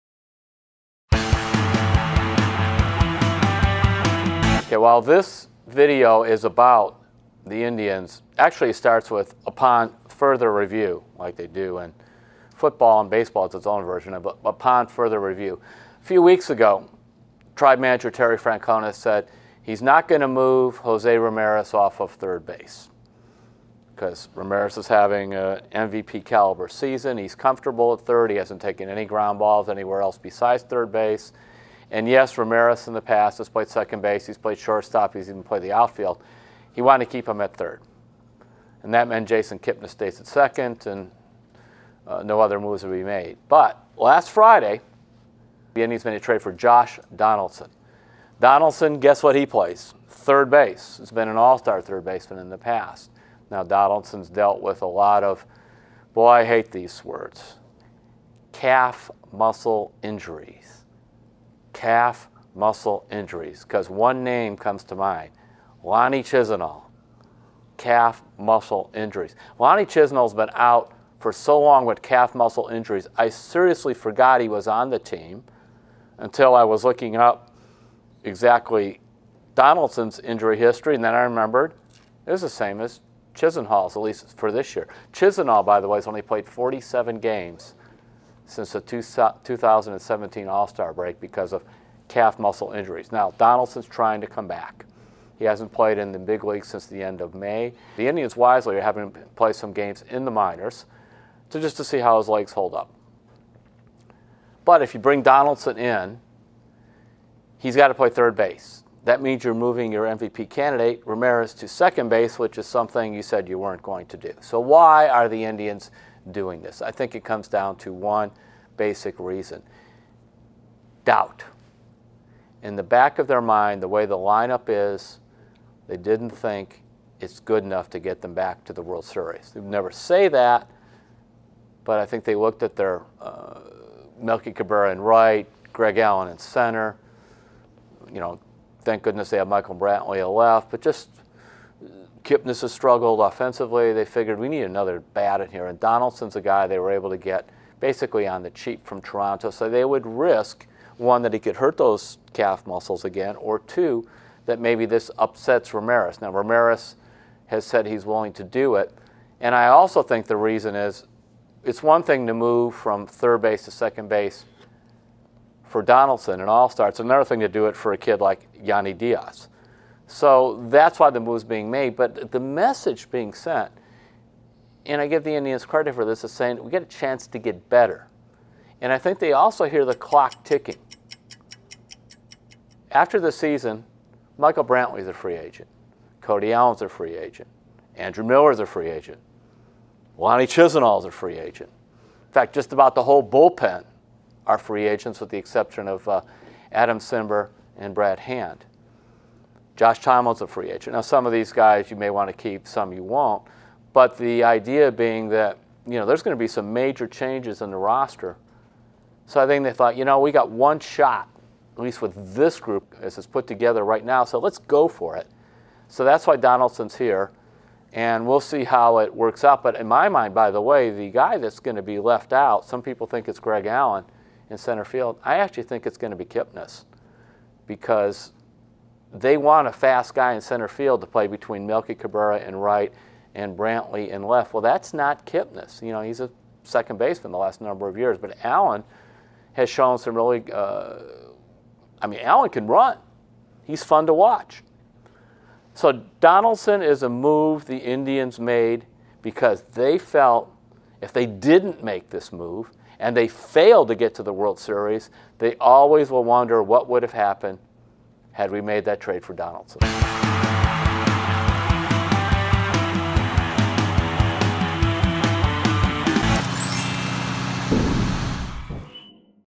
Terry Pluto is talking Cleveland Indians and Josh Donaldson
Terry Pluto says talks about the infield shakeup and some outfield adjustments.